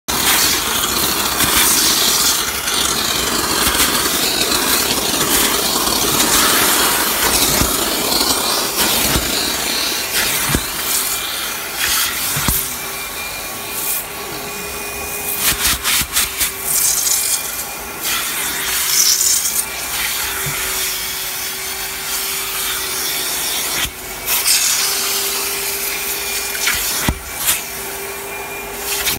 Aspirazione a vapore 160 gradi
Nel video si vede come il vapore scioglie lo sporco in abbinamento di prodotti professionali